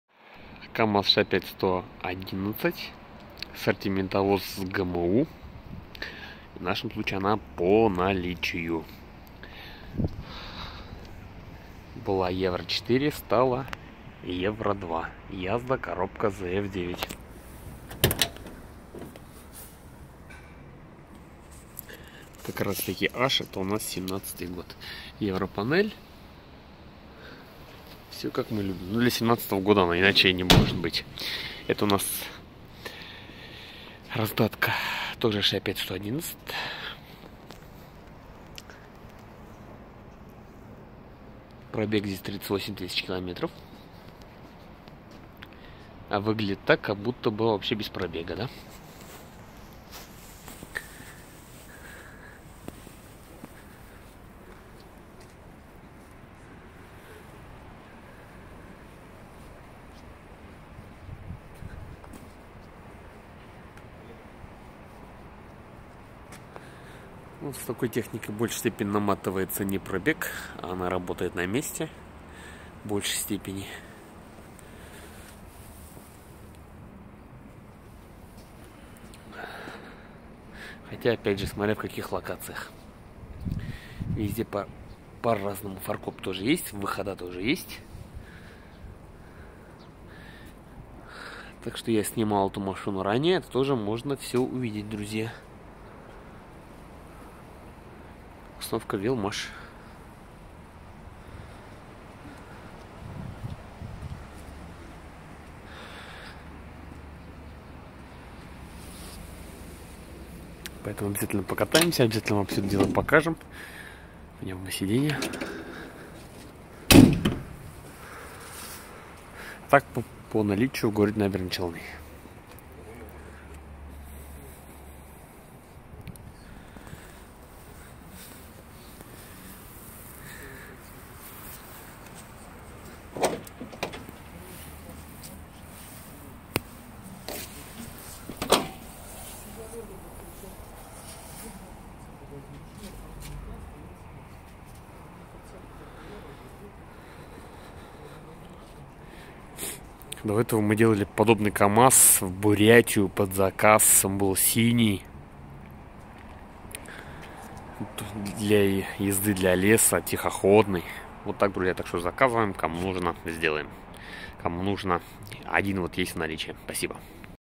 КамАЗ-65111 сортиментовоз с ГМУ 2017-го года в наличии
КамАЗ-65111, 2017, 38т.км, евро 2, язда, 300л.с, зф9💯🔥